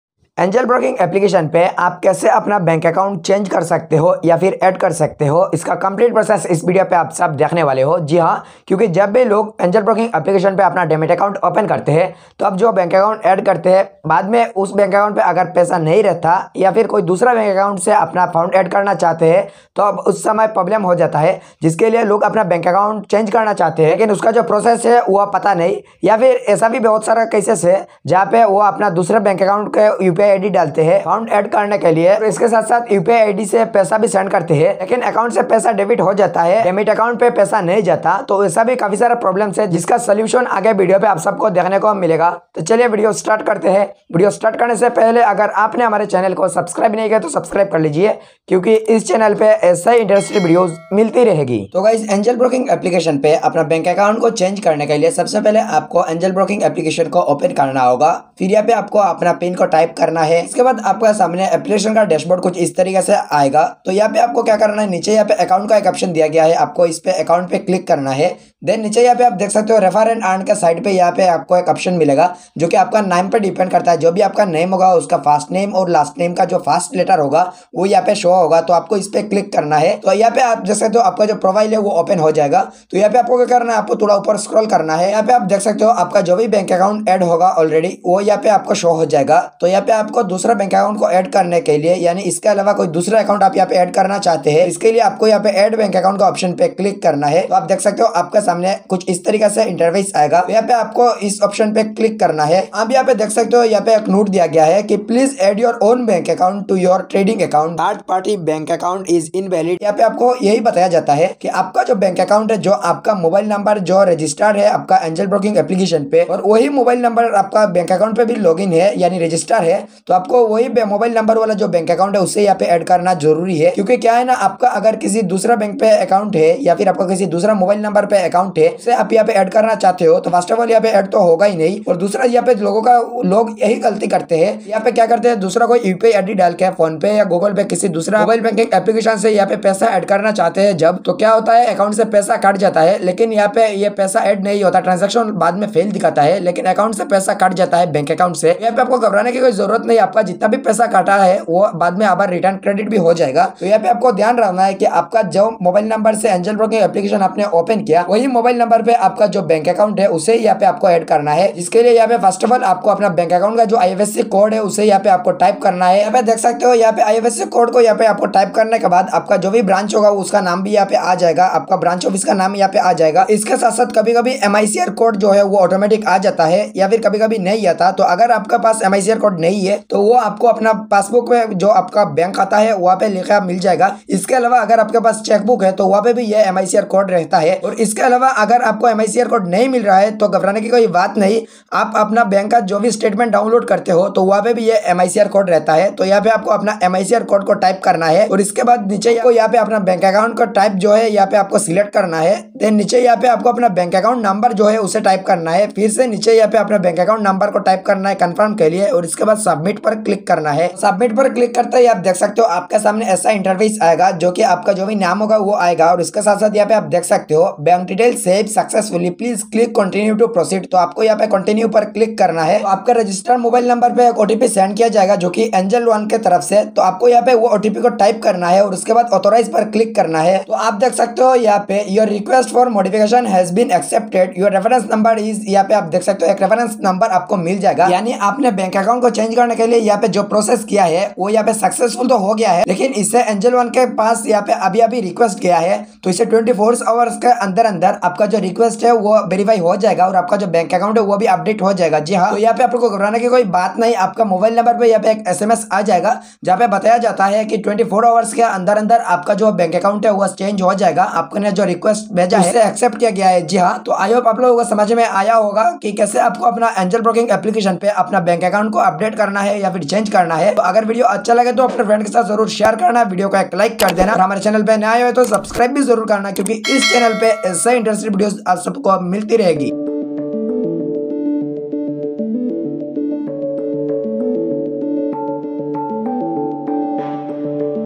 how to change bank account in Angel Broking | add bank account in demate account 2023 -Rajib Monitor
This video has been told about how to add or change bank account in Angel Broking application, often people are not able to add money after the bank account they add while opening demat account on Angel Broking application or they cannot add money to this account.  Also UPI link does not remain for which bank account has to be changed and how to change it is not known to everyone for which in this video I have seen how to add or change bank account on angel broking application.

My mic